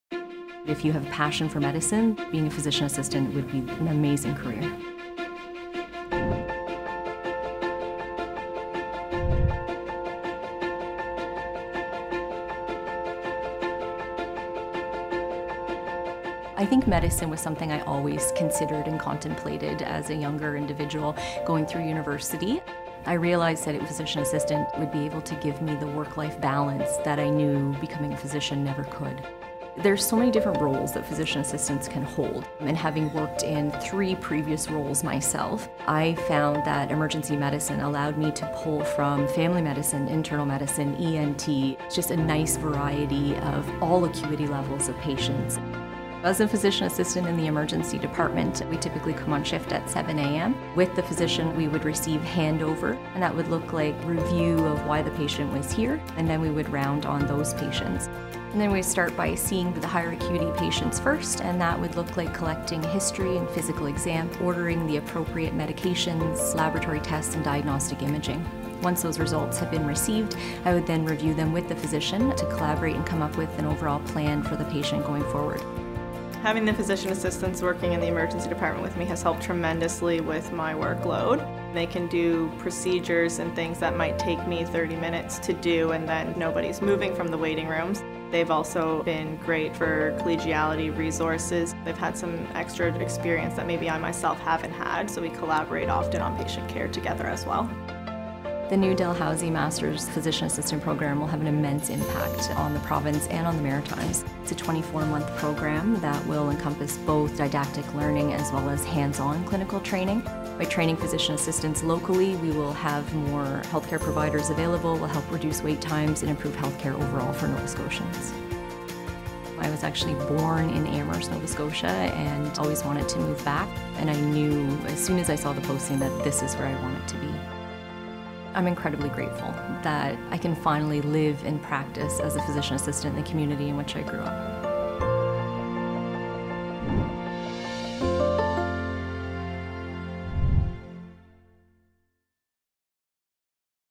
Action for Health: Physician Assistants
Physician Assistants are highly-trained medical professionals who work in all healthcare settings.

A new Physician Assistant program at Dalhousie University in Halifax began in January 2024